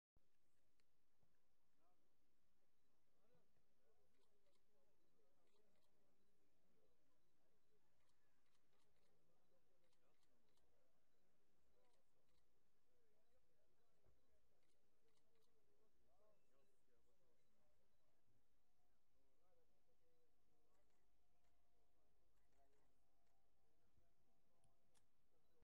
The scene of the fatal shooting in Wood...
Father-of-two killed in suspected drive by shooting outside a North London bakery.